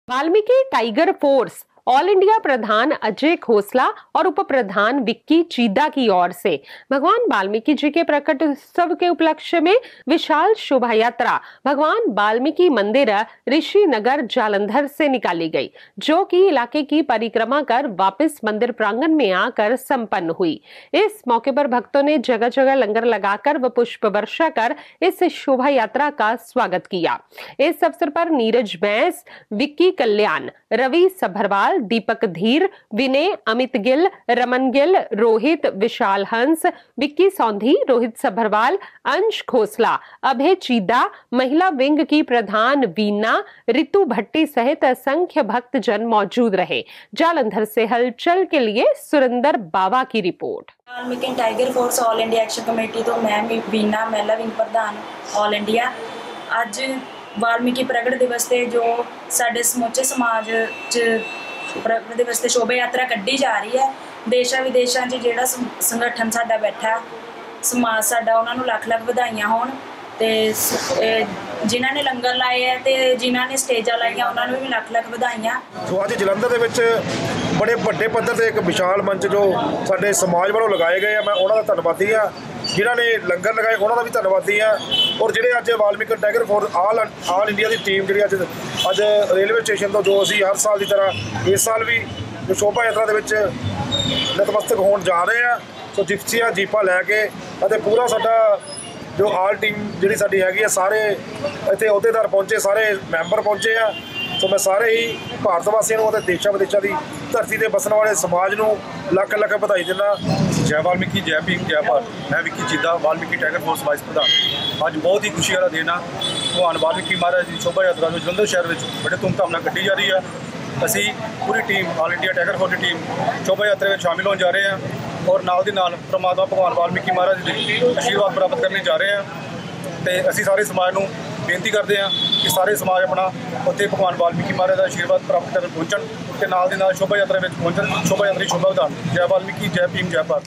Hulchul TV :वाल्मीकि टाईगर फोर्स ऑल इंडिया विशाल शोभायात्रा
वाल्मीकि टाईगर फोर्स ऑल इंडिया की ओर से भगवान वाल्मीकि जी के प्रकटोत्सव में विशाल शोभायात्रा